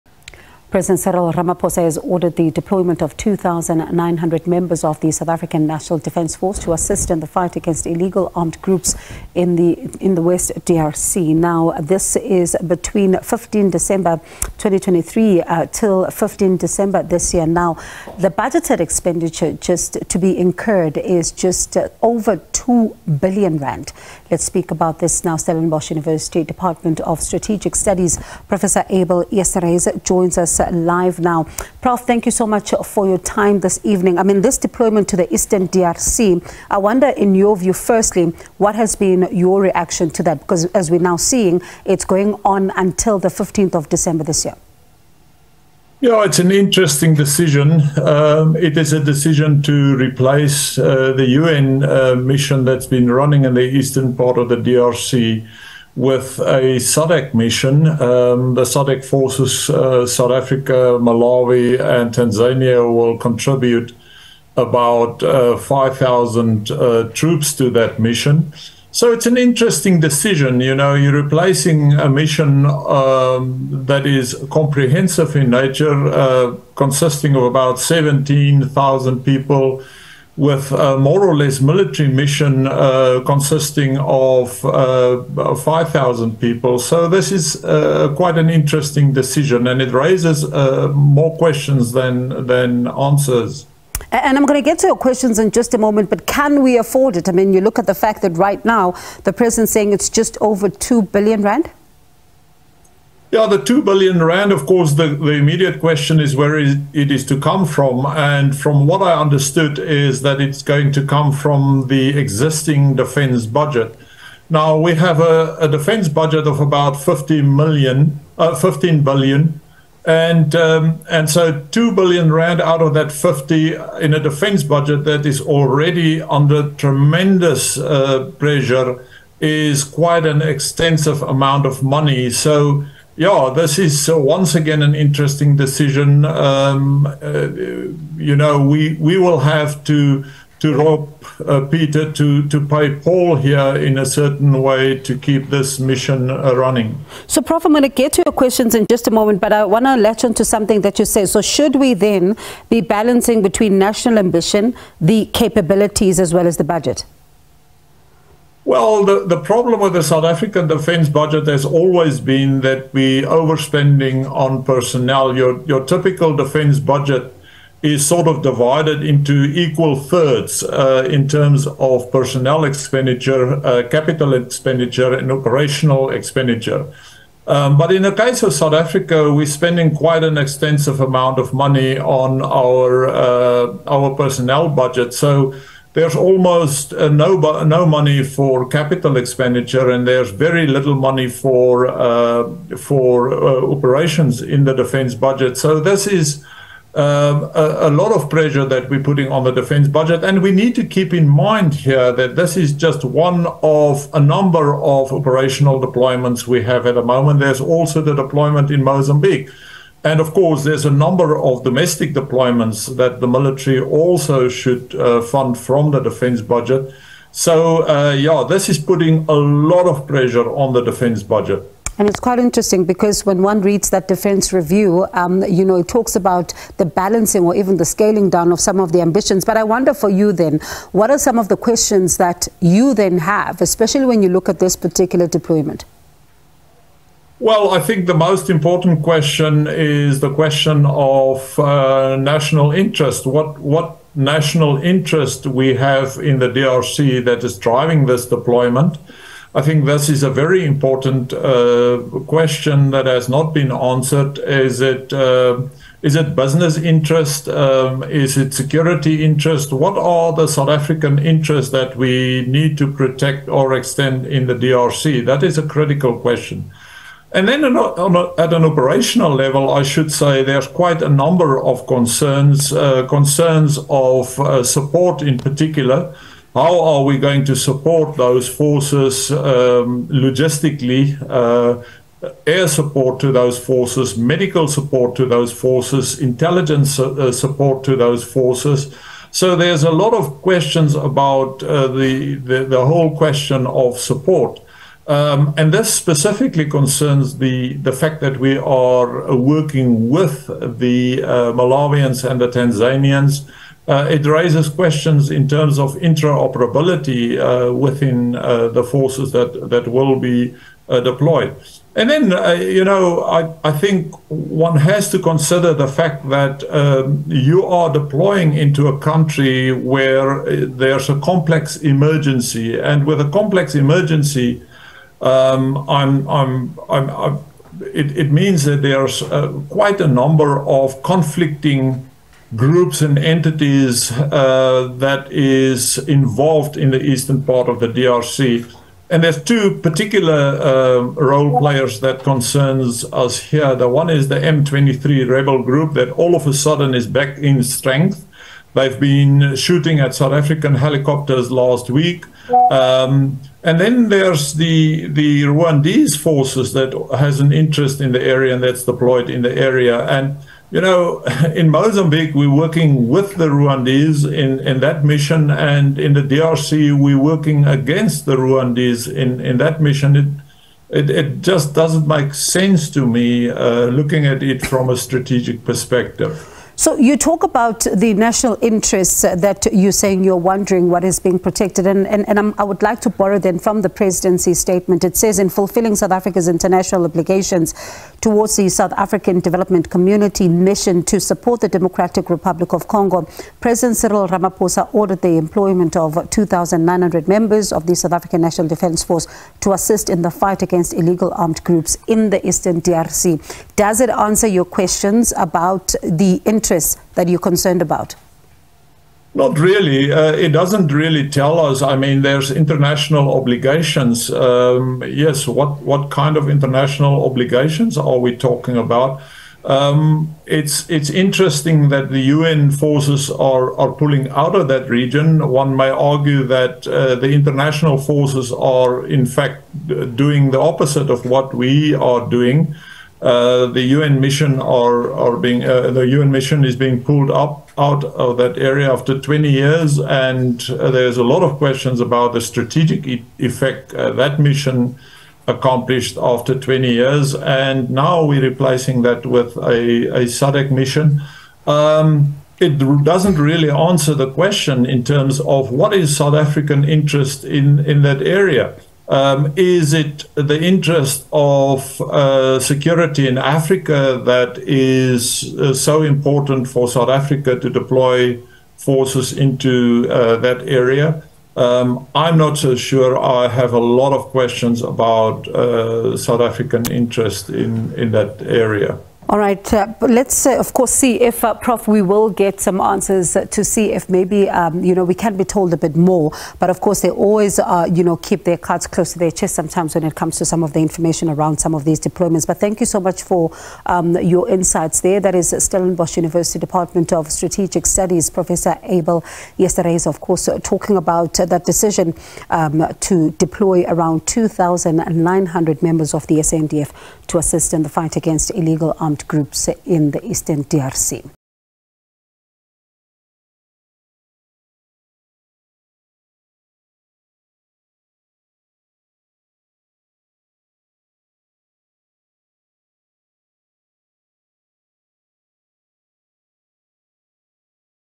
SA deploys troops to Eastern DRC: Prof Abel Esterhuyse weighs in
President Cyril Ramaphosa has ordered the deployment of 2900 members of the South African National Defence Force (SANDF) to assist in the fight against illegal armed groups in the Eastern DRC.

This is between 15 December 2023 and 15 December 2024. The budgeted expenditure to be incurred is just over R2 billion.

Stellenbosch University Department of Strategic Studies, Prof. Abel Esterhuyse joins the discussion.